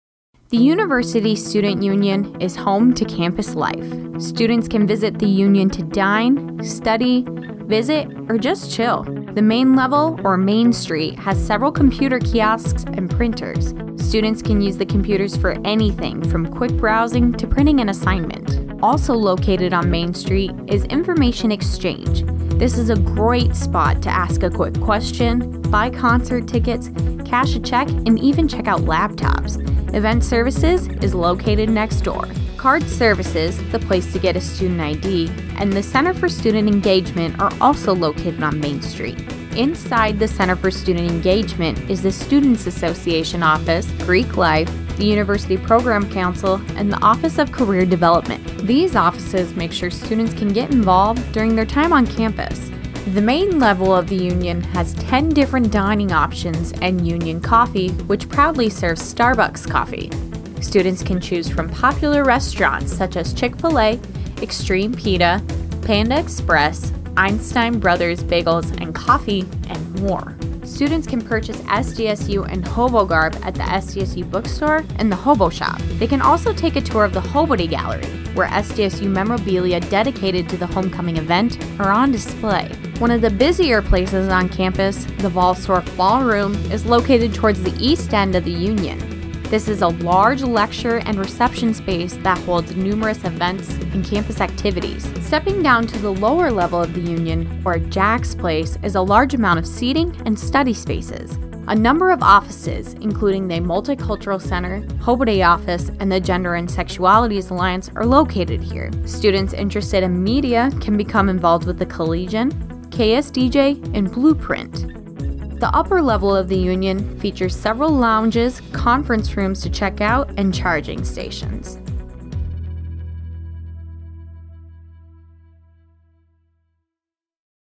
SDState Campus Tour | Student Union
The Student Union is home to campus life. Students can grab a bite to eat, shop at the University Bookstore, or buy concert tickets at Information Exchange. Take a look at what the Student Union has to offer!